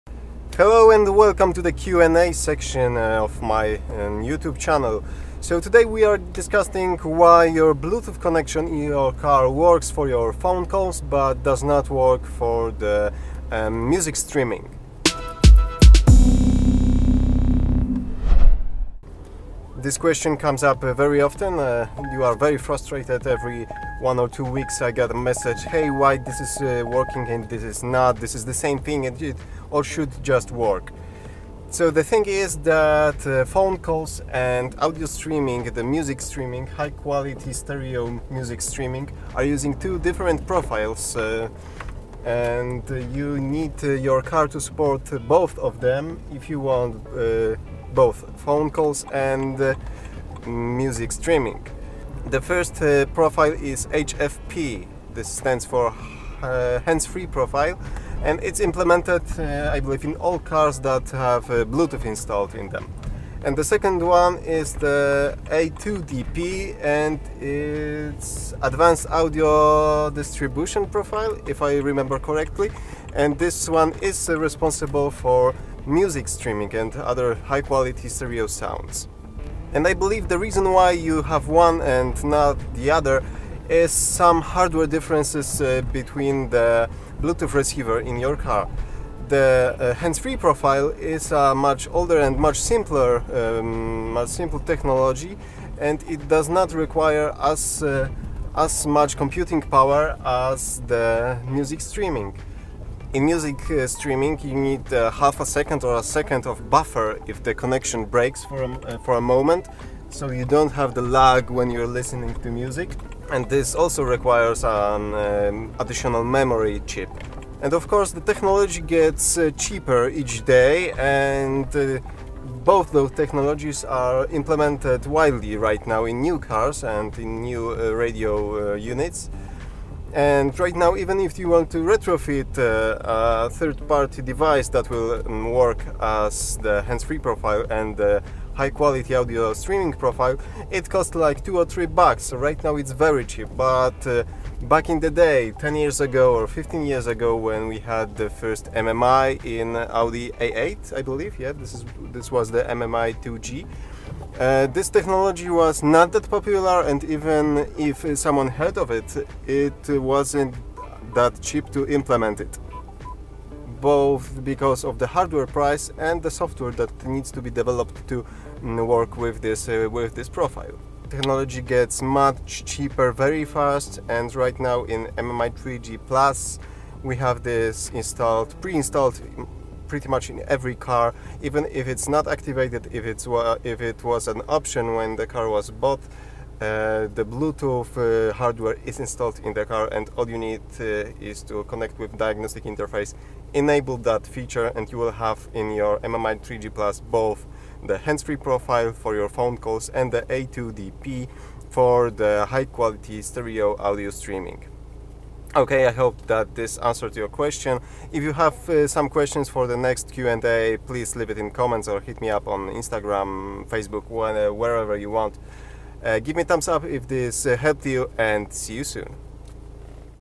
Why I can't play music though bluetooth in my car? A2DP vs HFP
To stream music via bluetooth to the stereo, your car needs to support A2DP (advanced audio distribution profile) standard which is not as common as the HFP (hands-free profile) used for phone-calls.
Even though both A2DP and HFP is using the same bluetooth connection, the A2DP requires more computing power and different software to support that feature.
Car media systems developed 15 years ago (like for example Audi MMI 2G) where not ready for such technology as it was very uncommon to have mobile device holding music that you want to play wirelessly over bluetooth. With time the mobile device market got much bigger and the hardware required for the A2DP support got much cheaper and easier to implement in the media system software.
Right now most car makers are including bluetooth receiver that supports both HFP and A2DP as standard equipment. And even if it's listed as option, most likely the hardware is in the car even if you didn't pay for it. It's only disabled with a configuration setting and can be enabled with a diagnostic interface (like in Audi MMI 3G and 3G+).